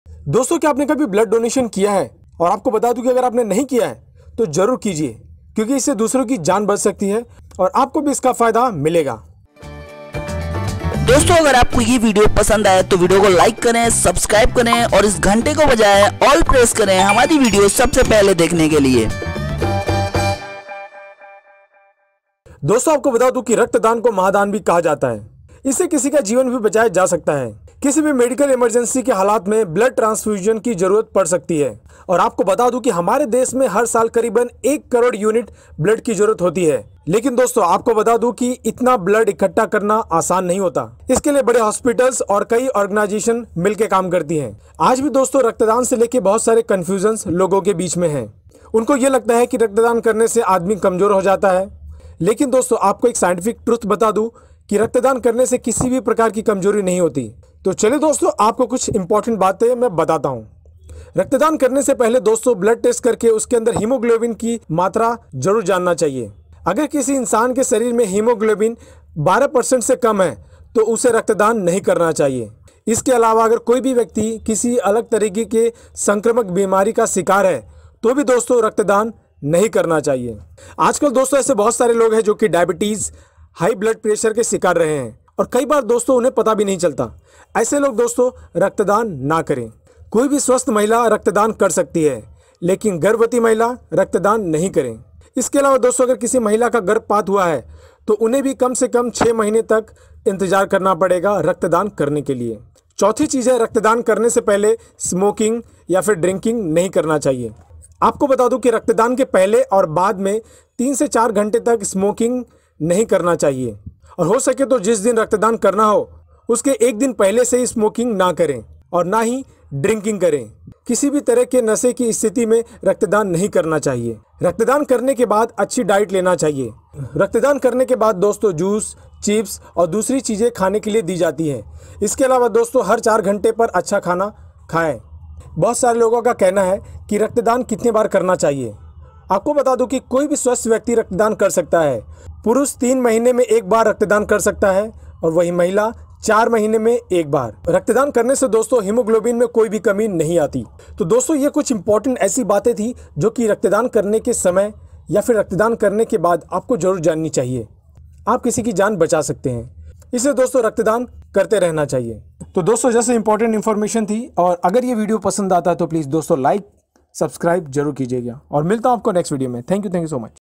जानें Blood Donation से जुड़ीं ये बातें, नहीं होगी कोई समस्या | रक्तदान से जुड़ी महत्वपूर्ण बातें।
NOTE- THIS VIDEO ONLY FOR INFORMATION .

1. ब्लड टेस्ट जरूरी
रक्तदान करने से पहले ब्लड टेस्ट कर उसमें हीमोग्लोबिन के स्तर को जानना जरूरी है। अगर किसी व्यक्ति का हीमोग्लोबिन 12 प्रतिशत से कम हो तो उसे रक्तदान नहीं करना चाहिए। इसके अलावा, अगर कोई व्यक्ति किसी तरह के संक्रमण का शिकार हो तो वह भी रक्तदान नहीं कर सकता है।

2. किसी तरह की बीमारी नहीं हो
आजकल बहुत से लोग डायबिटीज और हाई ब्लड प्रेशर के शिकार रहते हैं। कई बार तो उन्हें भी इसका पता नहीं होता। ऐसे लोगों को रक्तदान नहीं करना चाहिए।

3. गर्भवती महिलाएं
कोई भी स्वस्थ महिला रक्तदान कर सकती है, लेकिन गर्भवती महिलाओं को रक्तदान नहीं करना चाहिए। इसी तरह, जिन महिलाओं को गर्भपात हुआ हो, उन्हें भी कम से कम 6 महीने तक रक्तदान करने से बचना चाहिए।

4. रक्तदान के पहले स्मोकिंग नहीं करें
रक्तदान के पहले और बाद में 3-4 घंटे तक स्मोकिंग नहीं करना चाहिए। हो सके तो जिस दिन रक्दान करना हो, उसके एक दिन पहले से ही स्मोकिंग नहीं करें। किसी भी तरह के नशे की स्थिति में रक्तदान नहीं करना चाहिए।

5. अच्छी डाइट लें
रक्तदान करने के बाद अच्छी डाइट लेनी चाहिए। रक्तदान करने के बाद जूस, चिप्स और दूसरी चीजें खाने को दी जाती हैं। उनके अलावा भी हर 4 घंटे पर अच्छा खाना खाएं। जूस का सेवन करें।

6. कितनी बार कर सकते रक्तदान
कोई भी स्वस्थ व्यक्ति रक्तदान कर सकता है। पुरुष 3 महीने में एक बार रक्तदान कर सकते हैं, वहीं महिलाएं 4 महीने में रक्तदान कर सकती हैं। इससे हीमोग्लोबिन में कोई कमी नहीं आती।